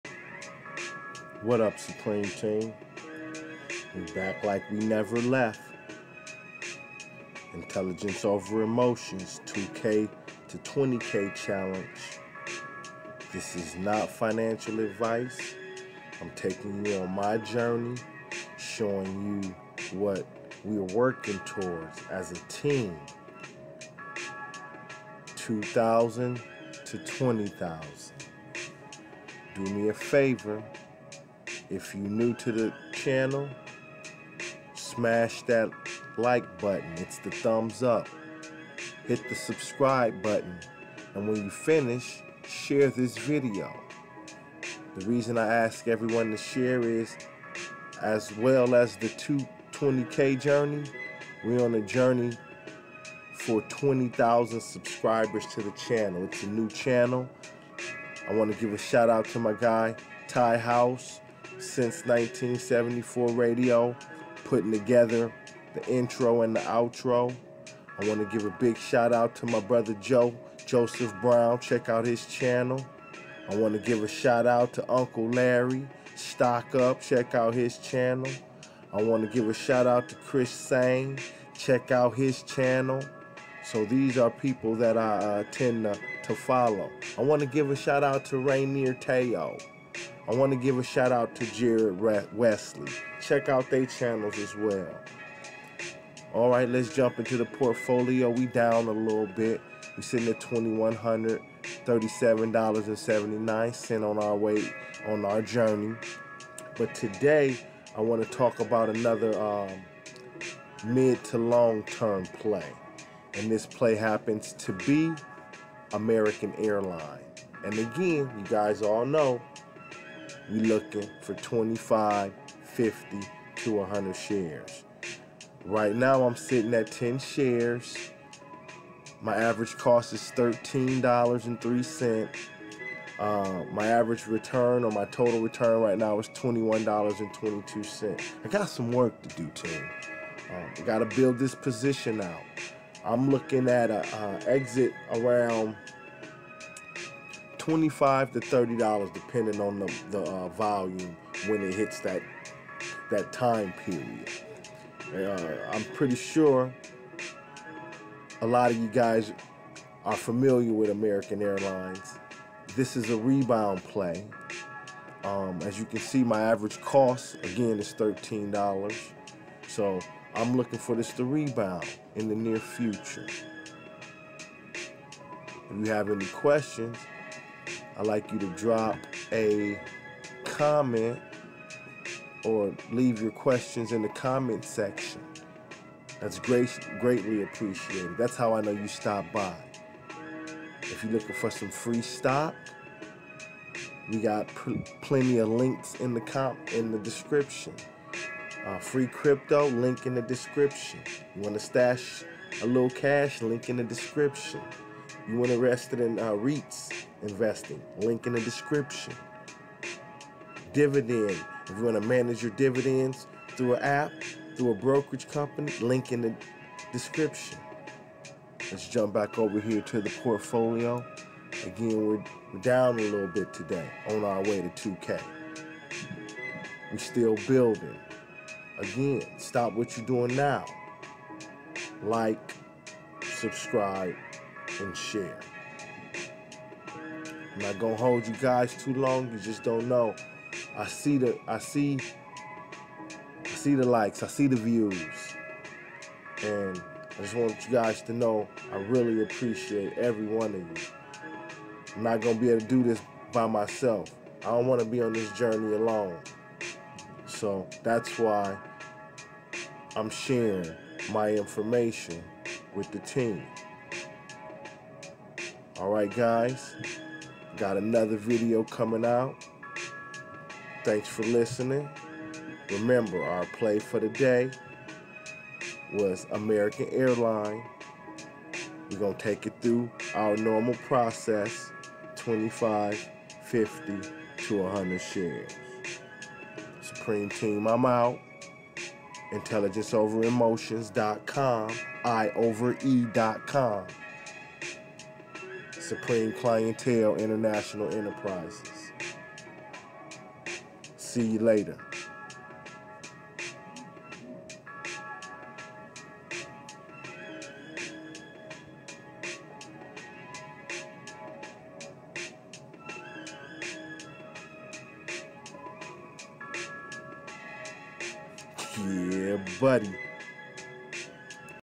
$2K to $20K Challenge "Money Talk - ($AAL)"
This is my introduction to the $2k to $20K challenge.  Come join us on the the journey and let's build together.